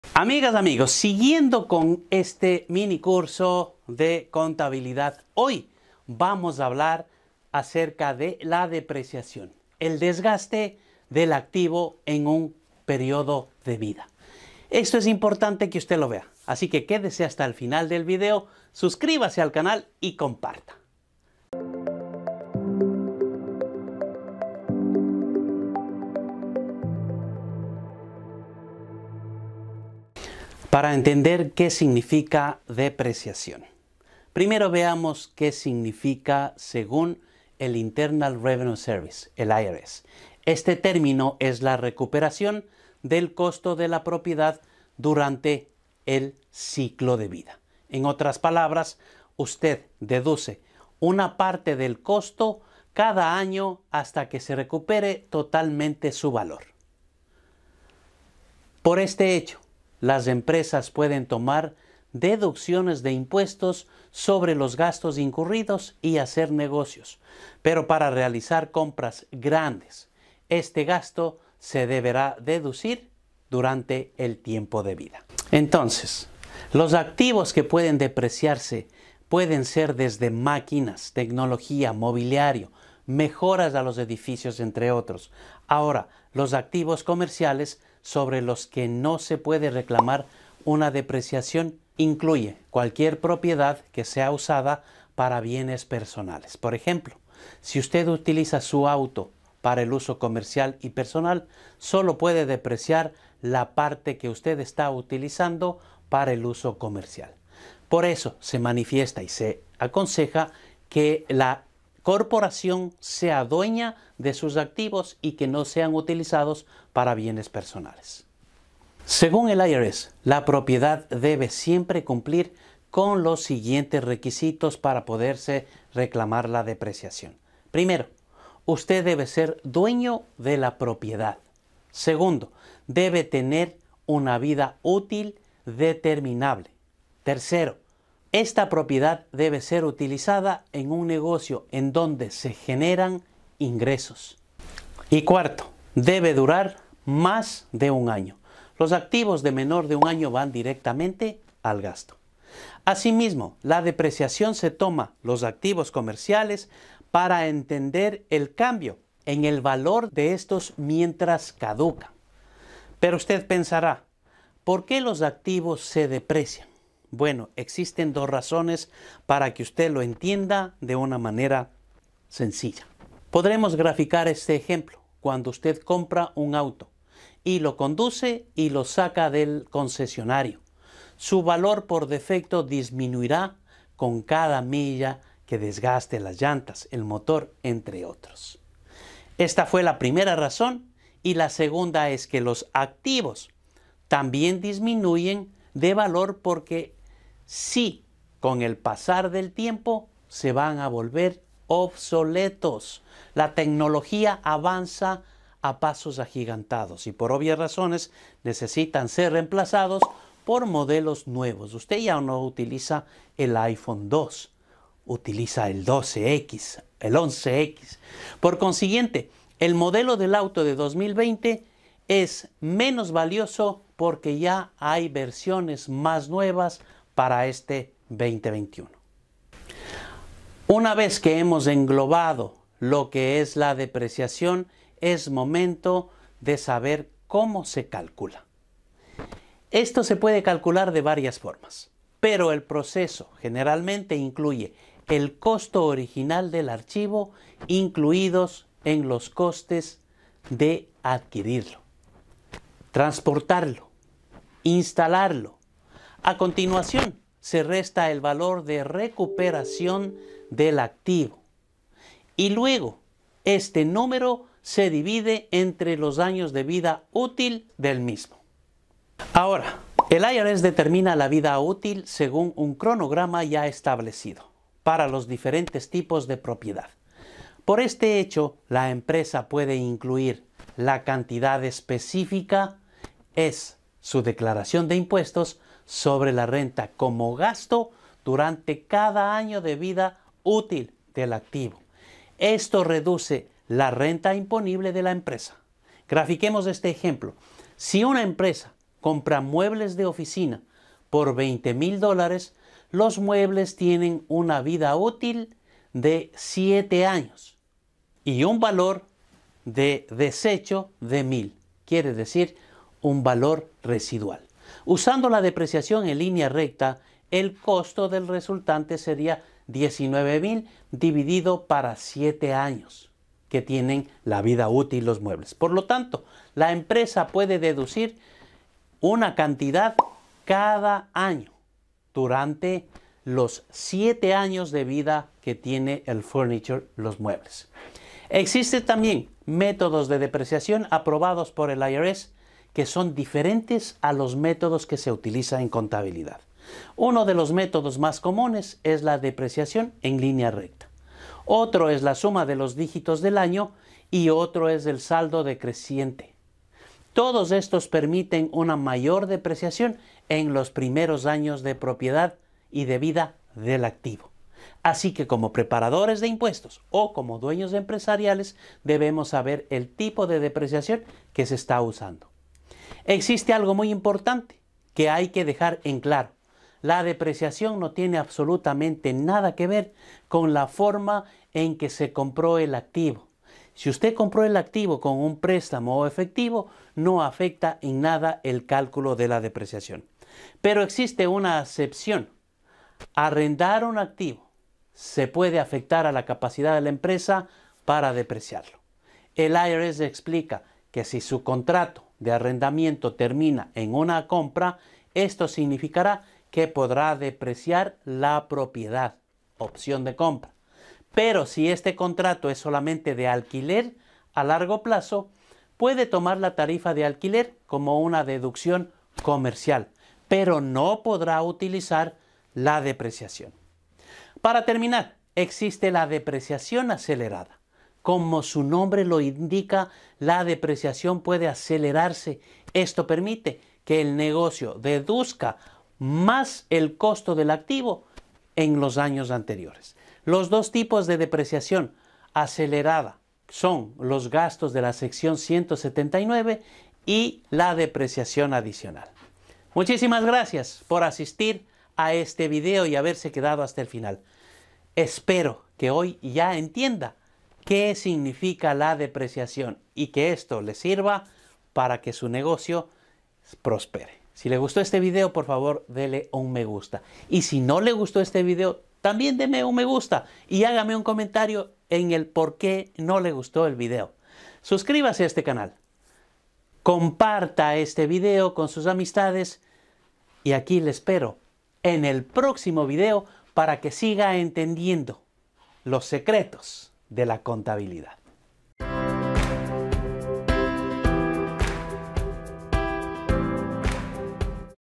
🟠¿SABE el SIGNIFICADO de DEPRECIACIÓN? | APRENDER CONTABILIDAD BÁSICA 2021 | Parte #6
En este video entenderemos lo que es la “Depreciación”, también analizaremos cómo se calcula y cómo el IRS trabaja con esta categoría. Si usted desea aprender más sobre este concepto no se pierda este video. Bienvenidos!